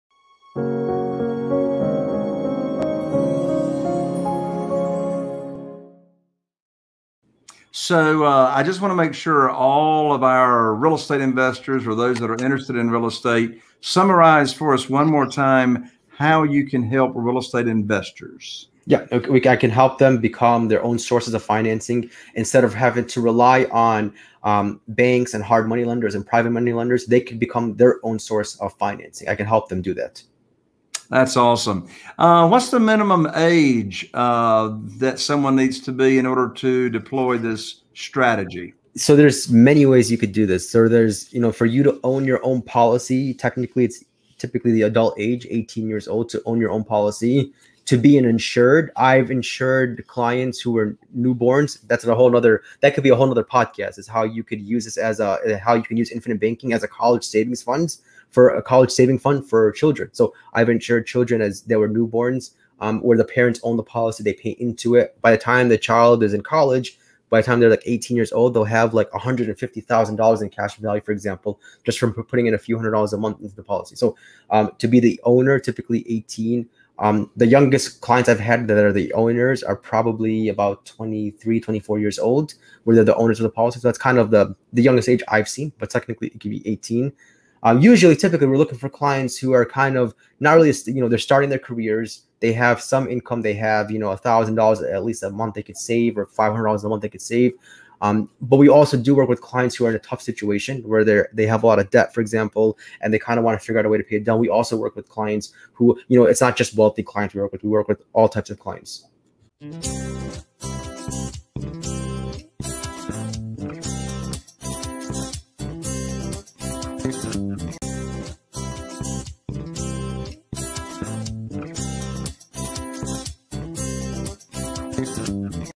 Become Your Own Source Of Financing with Sarry Ibrahim & Jay Conner, The Private Money Authority
“ I can help real estate investors become their own source of financing instead of having to rely on banks and hard money lenders.” - Sarry Ibrahim

Sarry Ibrahim is a financial planner and member of the Bank On Yourself Organization.

The Bank On Yourself Concept is a financial strategy that helps real estate investors, business owners, and full-time employees grow safe and predictable wealth regardless of market conditions.

Sarry started his financial services journey when he was in grad school completing his MBA. He worked for companies like Allstate, Blue Cross Blue Shield, Cigna Healthspring, and Human before founding Financial Asset Protection, a financial services firm that focuses on one sole concept; the Bank On Yourself Concept, also known as the Infinite Banking Concept.

For more valuable information click on this link and watch the complete episode - “Bank On Yourself: The Infinite Way Of Banking with Sarry Ibrahim & Jay Conner, The Private Money Authority”